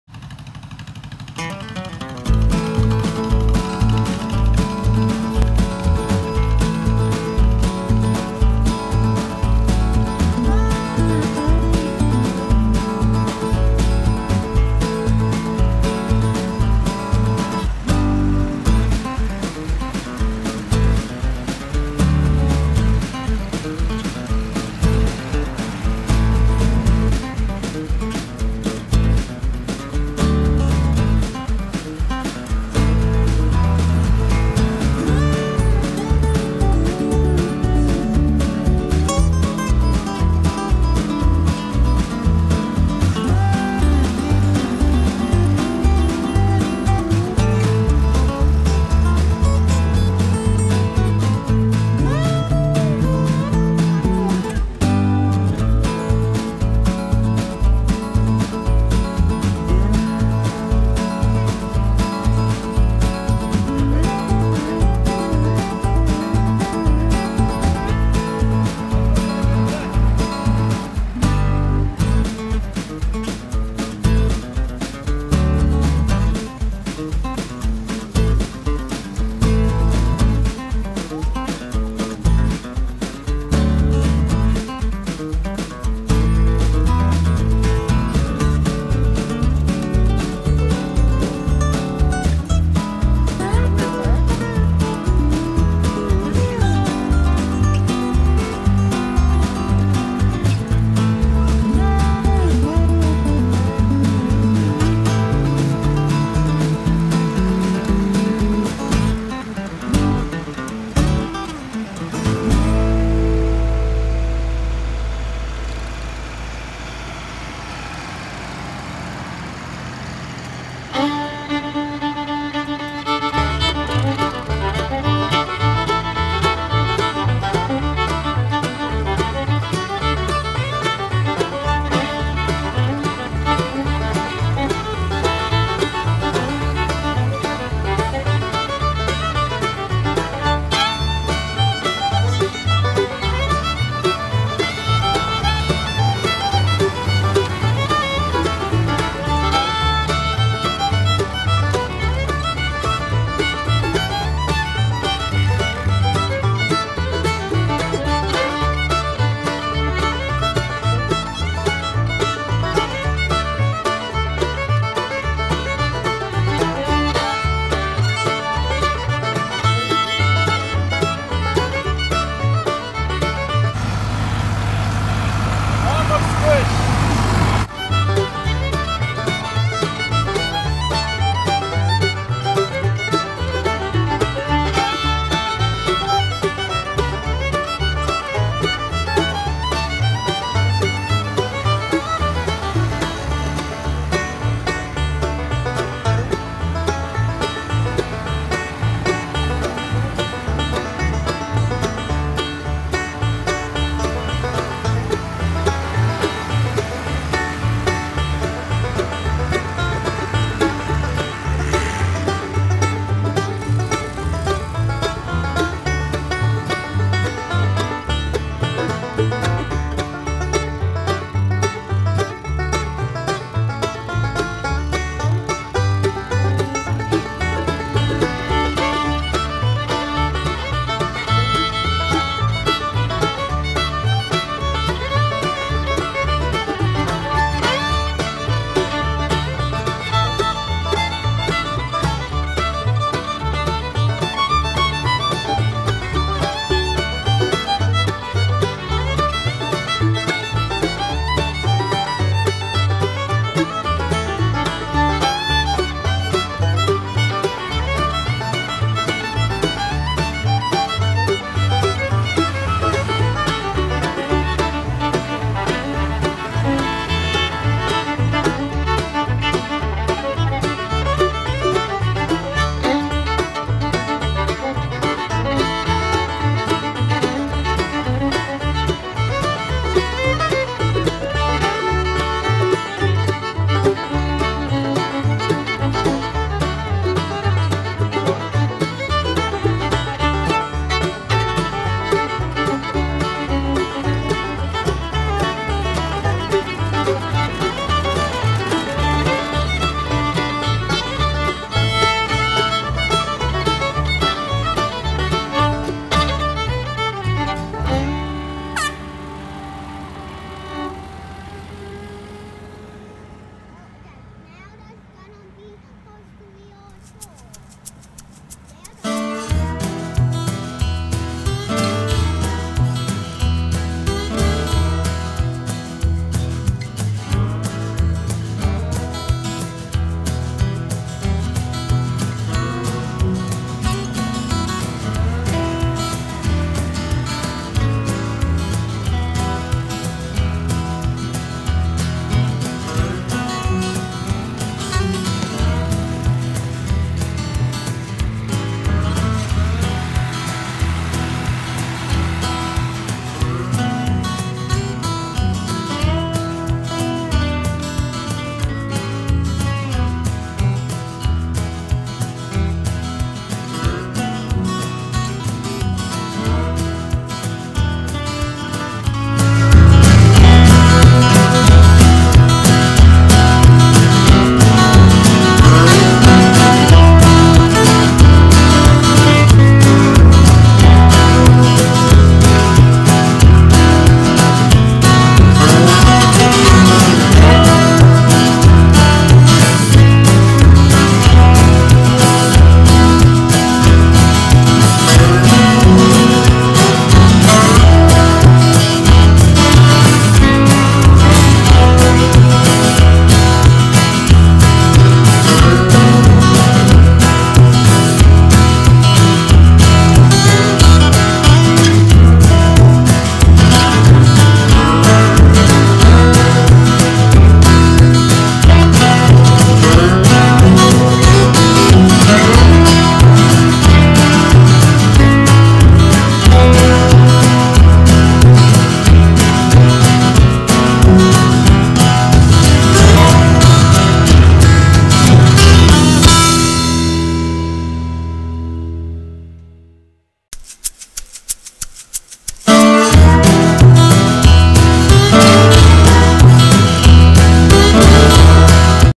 Antique tractor parade with extra pictures at the end
This is the annual “tractor trek” where tractor collectors get together and drive their tractors along  a specified parade route. Which takes them to “Smokin Coop” bbq pit right down the road from my house for lunch under the oak trees

I'm on Instagram as kenny_cooper_lawn_life. Install the app to follow my photos and videos.